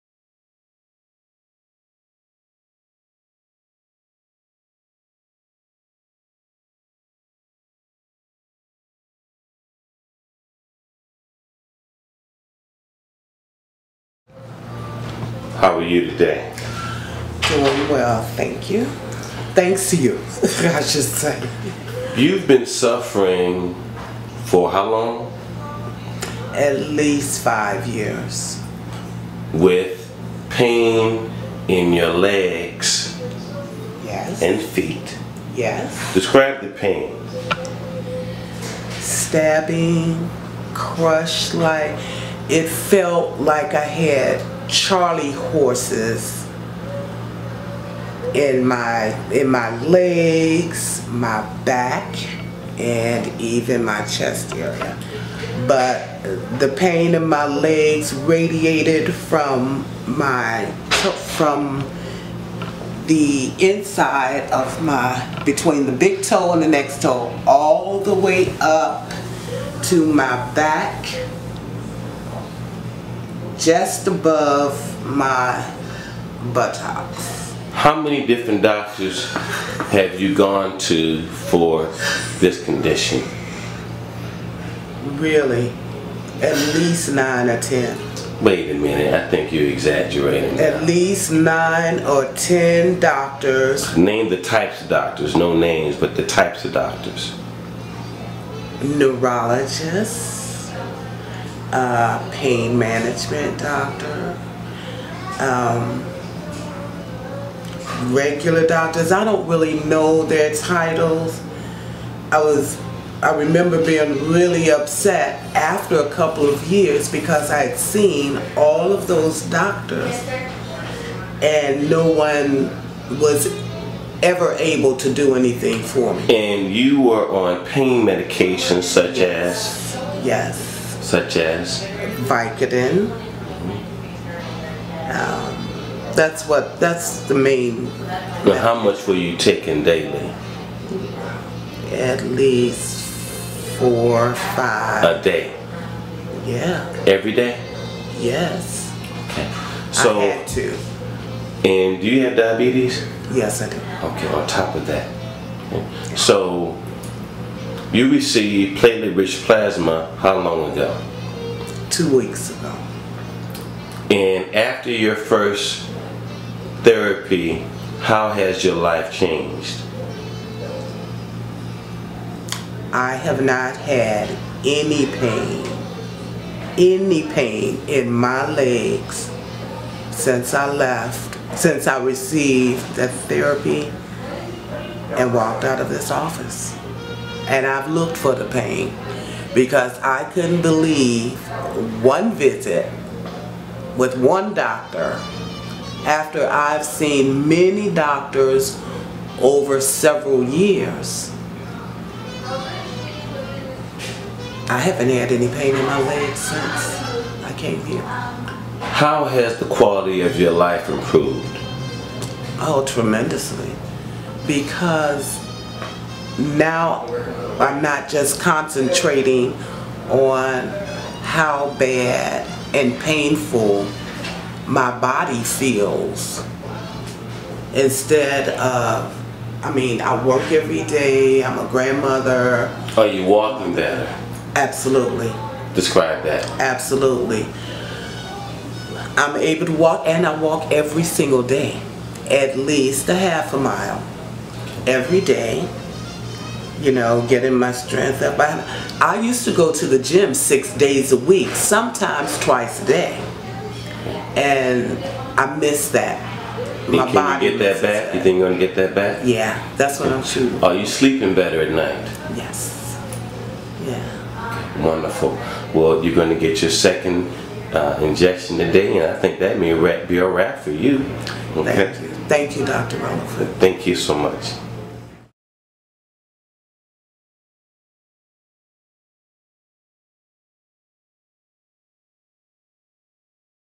Platelet-Rich-Plasma Injection (P.R.P.) | Los Angeles | Diabetic Neuropathy Treatment
Dr. Bill Releford, Medical Director of the Releford Foot and Ankle Institute, uses platelet rich plasma to treat and manage painful diabetic neuropathy.